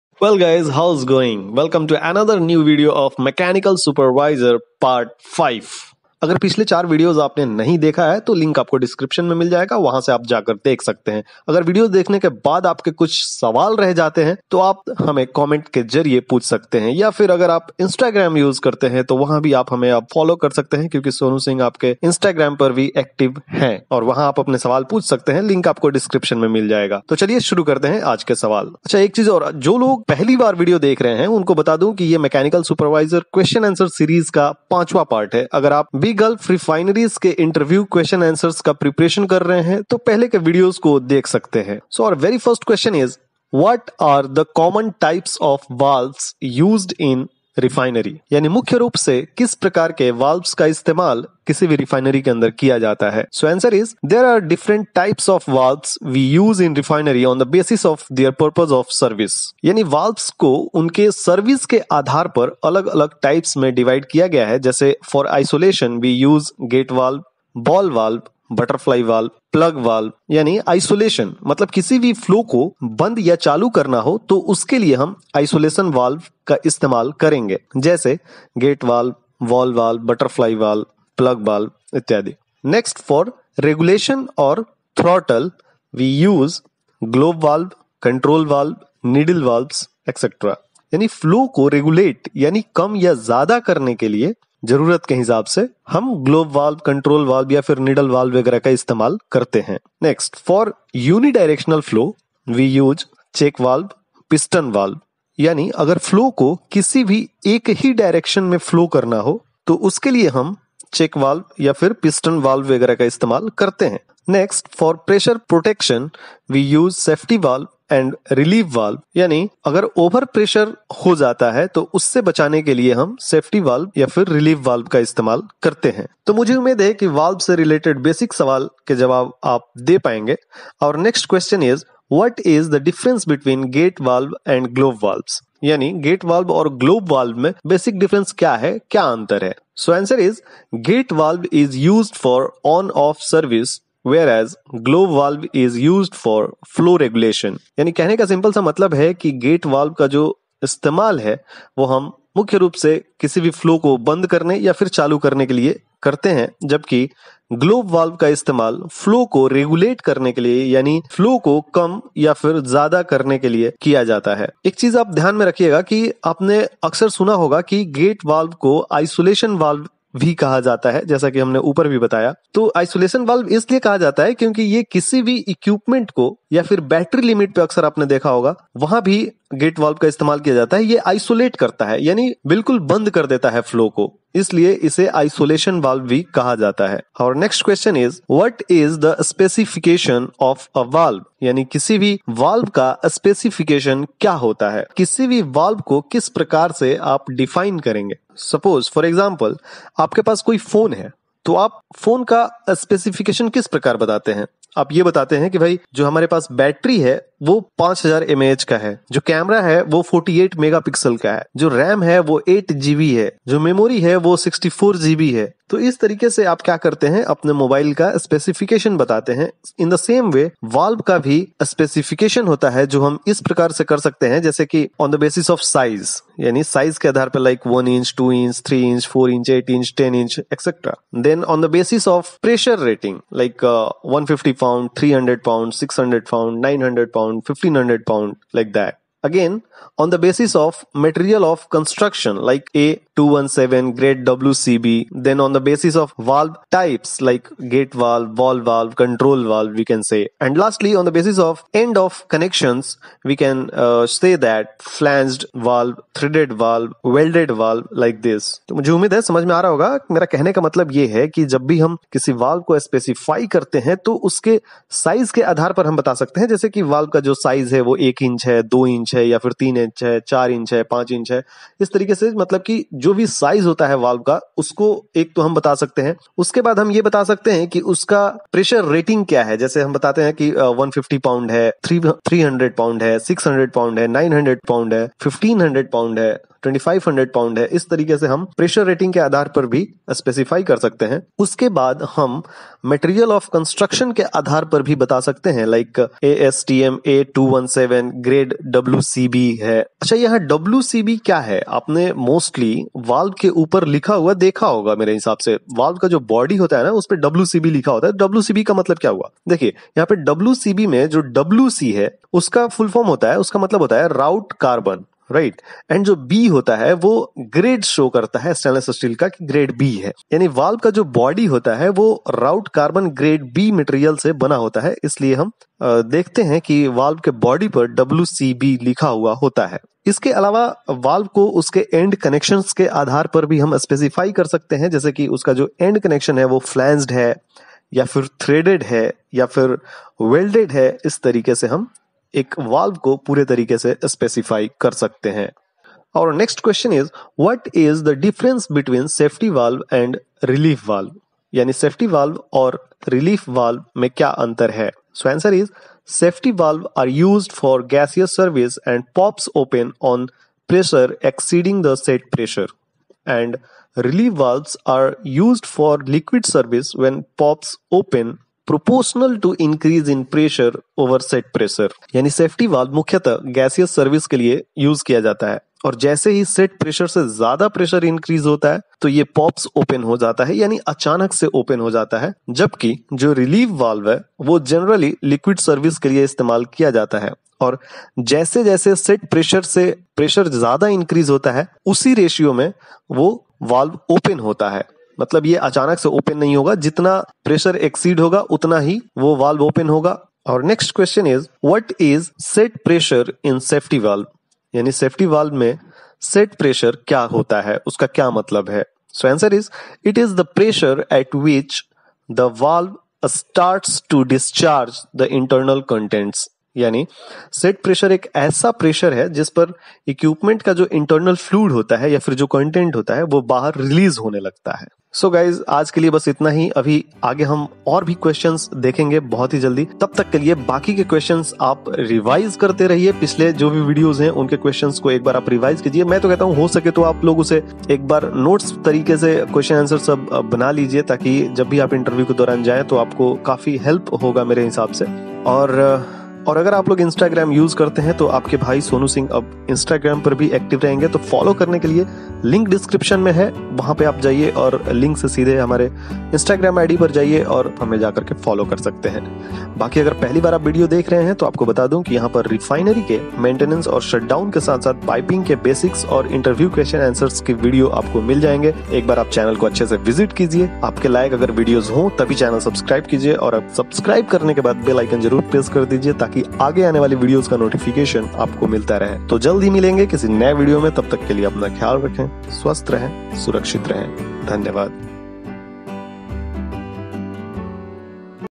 Part-05 | Mechanical Supervisor | Valve Questions | Shutdown Interview Questions
This is 5th part of maintenance supervisor (SMT) question answer series. In this video we discuss that how many gasket do we use in different types of shell & tube heat exchanger along with what is TEMA & BWG related questions and answers.